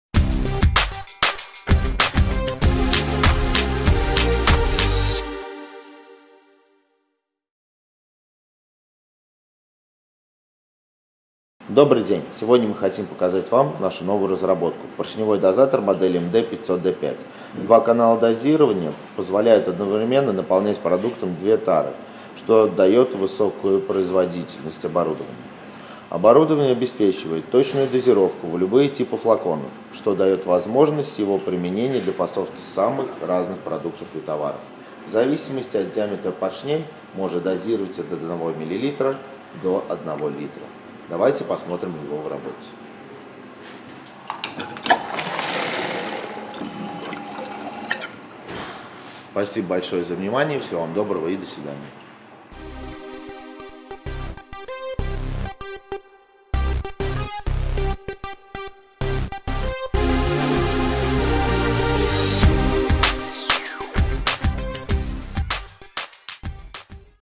Полуавтоматический настольный поршневой дозатор МД-500Д5
Показан принцип работы полуавтоматического поршневого дозатора МД-500Д5.

Дозирование является одним из ключевых этапов любого производства. От точности и равномерности распределения продукта зависит его качество и сохранность на протяжении заданного срока. Полуавтоматический настольный поршневой дозатор МД-500Д5 — одна из последних разработок специалистов «АВРОРА ПАК ИНЖИНИРИНГ». Оригинальная конструкция крановых распределителей обеспечивает широкие функциональные возможности установки, позволяющие дозировать практически любые продукты, в том числе горячие, полувязкие и пенящиеся. В производстве модели применяется высокопрочная нержавеющая сталь и другие качественные материалы зарекомендовавших себя поставщиков.

Назначение и область применения
Дозатор МД-500Д5 применяется для розлива жидких и пастообразных продуктов в стеклянную, пластиковую или металлическую тару.  Конструктивные возможности установки позволяют осуществлять розлив однородных веществ без абразивных частиц и вкраплений (лекарственные препараты, напитки, джемы, соусы, шампуни, крема, чистящие средства и другие продукты). Оборудование широко применяется на предприятиях пищевой, фармацевтической, косметической и химической промышленности.

Принцип работы
Оператор вручную устанавливает две единицы тары на позиции дозирования и запускает установку. Дозируемый продукт всасывается из расходной емкости и по гибким шлангам поступает в цилиндры, а затем — в установленные флаконы. После наполнения оператор меняет тару на следующую и повторяет процесс.

Основные элементы
 корпус (Д16Т /полипропилен)
 гильза-поршень (AISI 316L)
 электропривод (220 V /50 Гц).
Преимущества
 Высокая точность дозирования: погрешность дозы составляет не более 1%
 Два канала дозирования позволяют одновременно наполнять продуктом две тары, что обеспечивает высокую производительность оборудования
 Доза продукта регулируется от 100 до 450 мл
 Комплект сменных цилиндров позволяет изменять дозу фасовки в соответствии с производственными задачами
 Установка отличается простотой управления и обслуживания.
Универсальность
Оборудование обеспечивает точную дозировку в любые типы флаконов с открытой горловиной (банки, бутылки, стаканчики и т. д.), что обеспечивает возможность его применения для фасовки самых разных продуктов и товаров.

Удобство работы
Модель отличается эргономичной конструкцией, обеспечивающей удобство работы оператора. Управление осуществляется при помощи кнопок.

Варианты исполнения
Исполнение данного дозатора определяется исходя из специфики производства Заказчика, особенностей продукта и индивидуальных производственных задач. Для обеспечения максимальной точности розлива малых доз продукта установка комплектуется специальным раздаточными патрубками и расходной емкостью. Для работы в условиях повышенной пожарной опасности оборудование может быть изготовлено во взрывозащищенном исполнении.

Безопасность
На всех этапах производства данного оборудования проводится многоступенчатый контроль качества, что гарантирует надежность и безопасность дозатора при его эксплуатации.